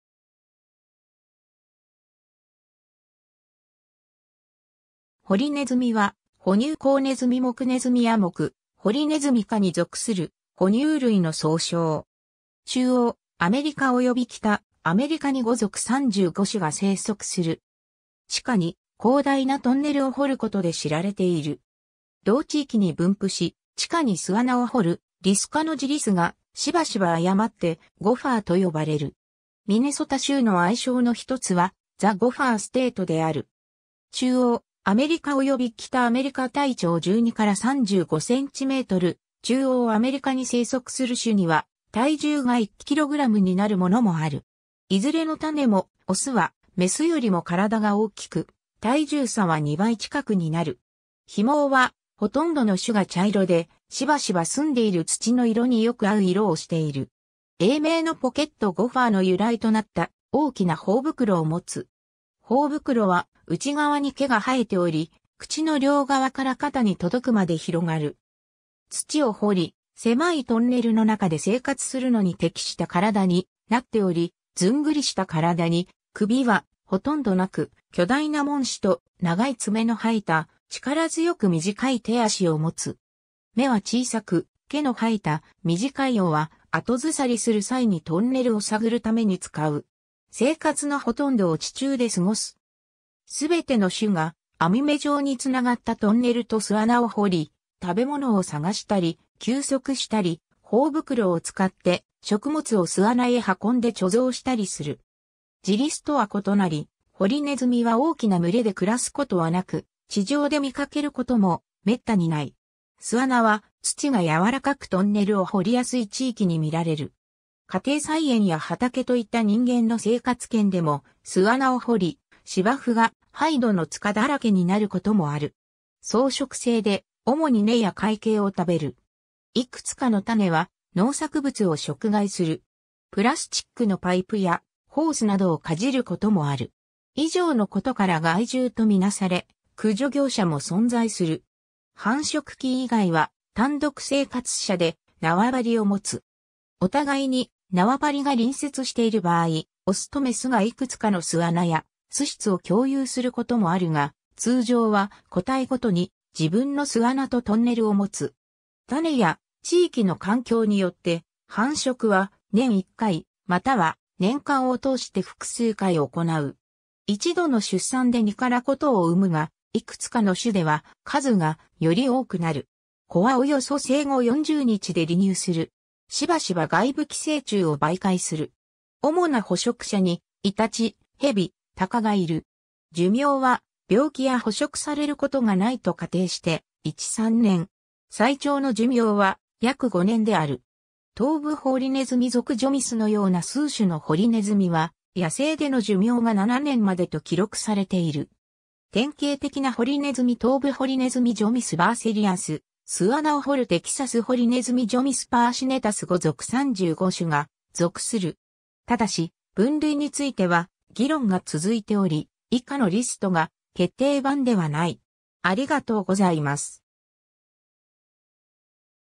ホリネズミ
ホリネズミ (英: Gopher)は、哺乳綱ネズミ目（齧歯目）ネズミ亜目ホリネズミ科に属する哺乳類の総称。
 中央アメリカおよび北アメリカに5属35種が生息する。
 地下に広大なトンネルを掘ることで知られている。
 同地域に分布し、地下に巣穴を掘るリス科のジリスが、しばしば誤って gopher と呼ばれる。
 ミネソタ州の愛称のひとつは、"the Gopher State"（ホリネズミの州）である。
 中央アメリカおよび北アメリカ（固有種） 体長12-35センチメートル、中央アメリカに生息する種には、体重が1キログラムになるものもある。
 いずれの種も、オスはメスよりも体が大きく、体重差は2倍近くになる。
 被毛は、ほとんどの種が茶色で、しばしば住んでいる土の色によく合う色をしている。
英名の "pocket gopher" の由来となった、大きな頬袋を持つ。
 頬袋は、内側に毛が生えており、口の両側から肩に届くまで広がる。
 土を掘り、狭いトンネルの中で生活するのに適した体になっており、ずんぐりした体に、首はほとんどなく、巨大な門歯と、長い爪の生えた、力強く短い手足を持つ。
 目は小さく、毛の生えた短い尾は、後ずさりする際にトンネルを探るために使う。
 生活のほとんどを地中で過ごす。
すべての種が、網目状につながったトンネルと巣穴を掘り、食べ物を探したり、休息したり、頬袋を使って食物を巣穴へ運んで貯蔵したりする。
ジリスとは異なり、ホリネズミは大きな群れで暮らすことはなく、地上で見かけることも滅多にない。
巣穴は、土が柔らかくトンネルを掘りやすい地域に見られる。
家庭菜園や畑といった人間の生活圏でも巣穴を掘り、芝生が排土の塚だらけになることもある。
 草食性で、主に根や塊茎を食べる。
いくつかの種は農作物を食害する。
プラスチックのパイプやホースなどをかじることもある。
以上のことから害獣と見なされ、駆除業者も存在する。
 繁殖期以外は単独生活者で、縄張りを持つ。
お互いに縄張りが隣接している場合、オスとメスがいくつかの巣穴や巣室を共有することもあるが、通常は、個体ごとに自分の巣穴とトンネルを持つ。
種や地域の環境によって、繁殖は年1回、または年間を通して複数回行う。
1度の出産で2-5頭を産むが、いくつかの種では数がより多くなる。
子はおよそ生後40日で離乳する。
 しばしば外部寄生虫を媒介する。
 主な捕食者に、イタチ、ヘビ、タカがいる。
 寿命は、病気や捕食されることがないと仮定して、1−3年。
 最長の寿命は約5年である。
トウブホリネズミ属 Geomys のような数種のホリネズミは、野生での寿命が7年までと記録されている。
 典型的なホリネズミ トウブホリネズミ Geomys bursarius 巣穴を掘るテキサスホリネズミ Geomys personatus 5属35種が属する。
ただし、分類については議論が続いており、以下のリストが決定版ではない。